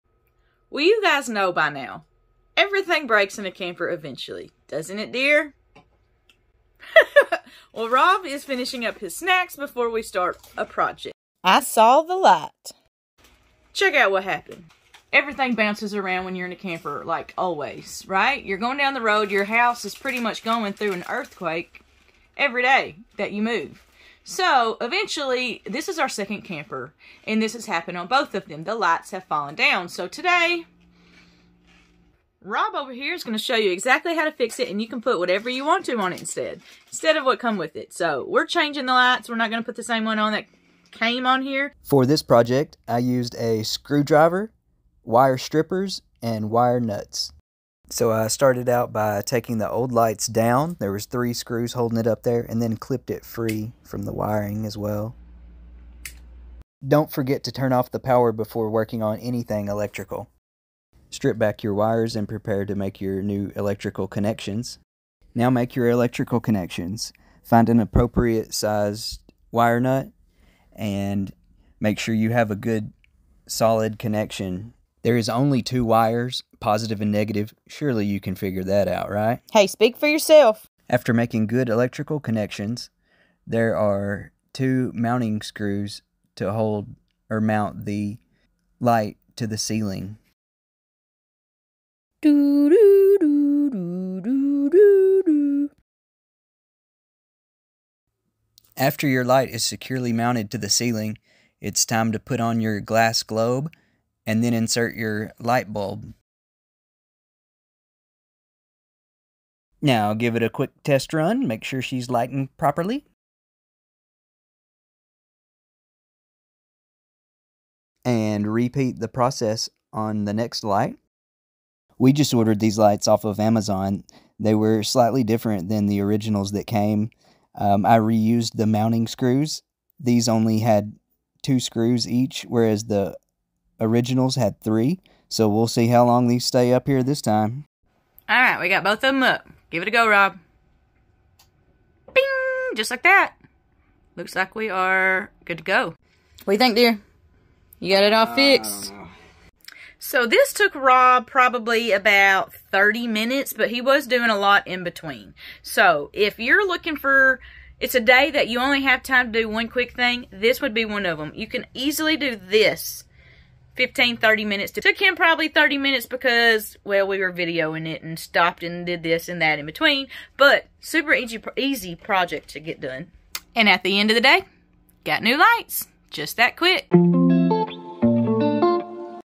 RV camper light fixture replacement tutorial in 3 minutes
When everything breaks in RVs, it's good to be handy. Here's a quick 3-minute time-lapse video with detailed instructions to help replace your falling lights.